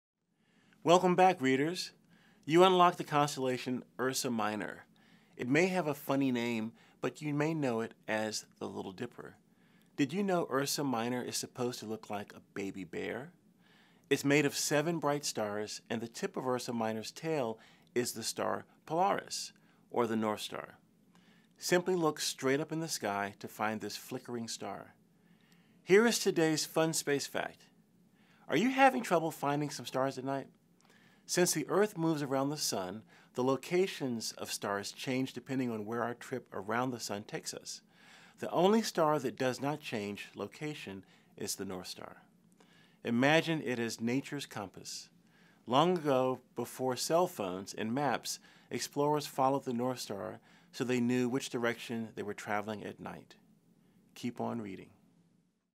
Ursa Minor: Summer Reading Challenge Constellation 9
Kids have unlocked the ninth reading milestone of the Scholastic Summer Reading Challenge! Watch as NASA Astronaut Leland Melvin shares fun facts about Ursa Minor and how to locate it in the night sky.